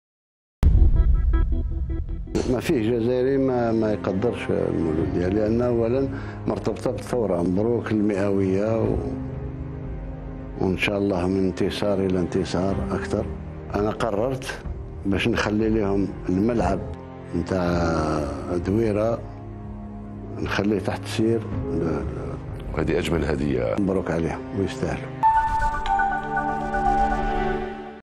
الرئيس تبون يضع ملعب الدويرة تحت تصرف فريق مولودية الجزائر
الجزائر (وأج) - قرّر رئيس الجمهورية عبد المجيد تبون وضع ملعب الدويرة، تحت تصرف مولودية الجزائر بمناسبة الذكرى المئوية لتأسيسه وقال في لقائه الدوري مع وسائل الإعلام الوطنية، أنه لا يوجد جزائري لا يقدّر فريق المولودية.